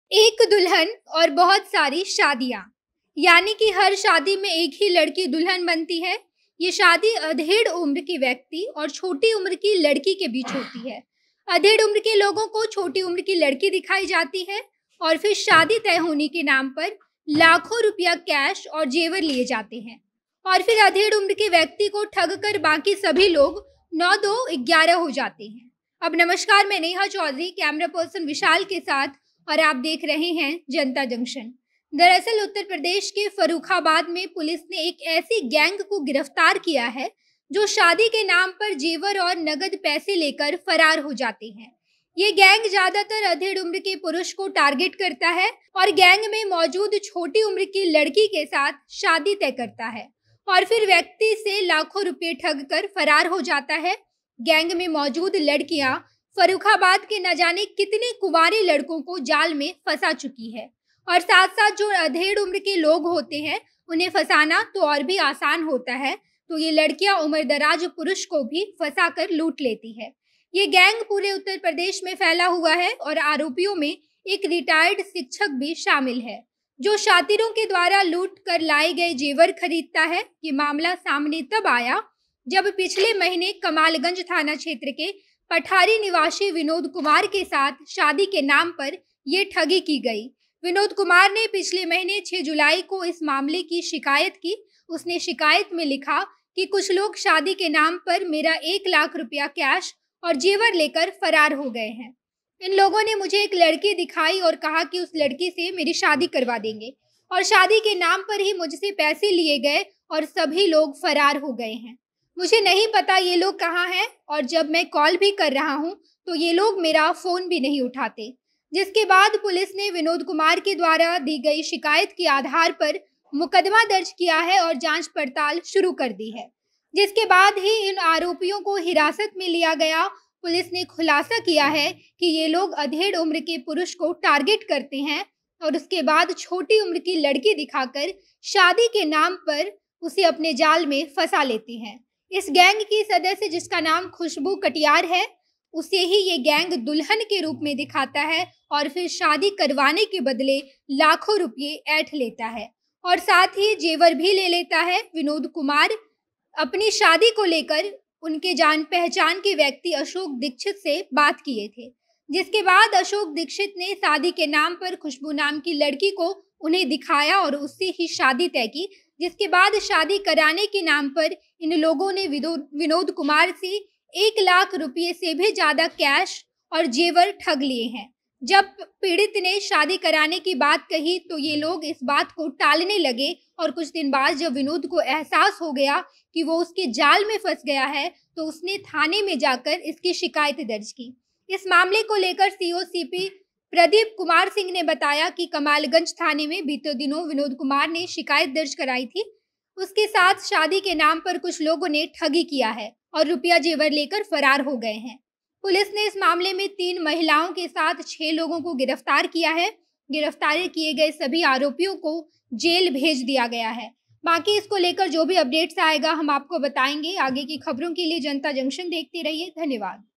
UP की ये लड़की, बूढ़ों को अपने जाल में फंसाती है, फिर लाखों रुपए और जेवर लेकर फरार हो जाती है!
उत्तर प्रदेश का यह गिरोह बूढ़े पुरुष की छोटी उम्र की लड़की से शादी लगाता है, फिर अपने जाल में फंसा कर लाखों रुपए और जेवर लेकर फरार हो जाता है!